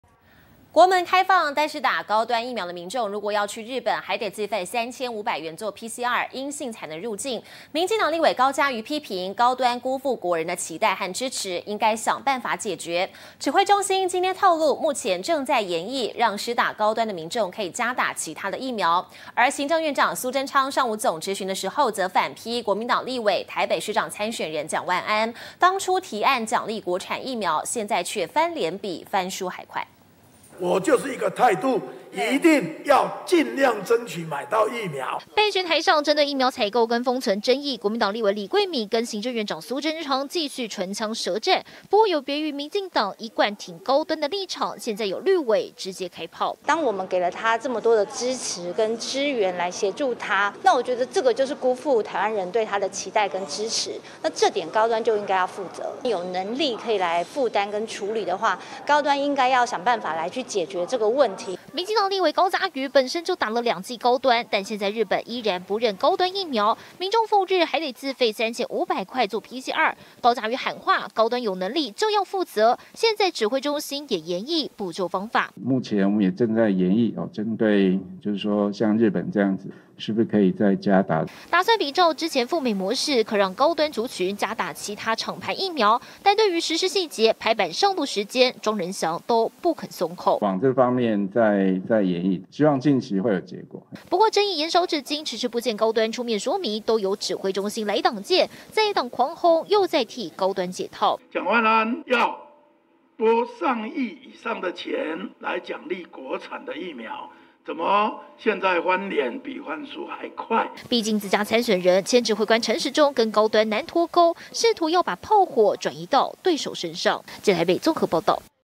打高端赴日需自費PCR! CDC:研議補打其他疫苗
國門開放，但打高端疫苗的民眾如果要去日本，還得自費3500元做PCR，陰性才能入境。民進黨立委高嘉瑜批評，高端辜負國人的期待和支持，應該想辦法解決，指揮中心今天(18號)透露，目前正在研議，讓施打高端的民眾，可加打其他疫苗。行政院長蘇貞昌上午總質詢時，則反批國民黨立委、台北市長參選人的蔣萬安，當初提案獎勵國產疫苗，現在卻翻臉比翻書還快。